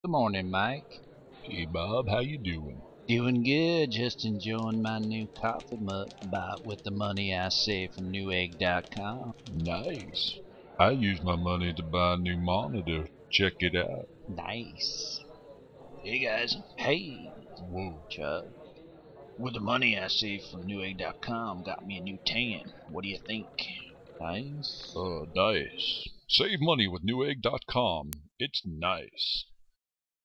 newegg eggstreme campus challenge video contest
3d Animation for newegg.com. Made on the computer purchased from Newegg. Thanks Newegg for allt he money I saved!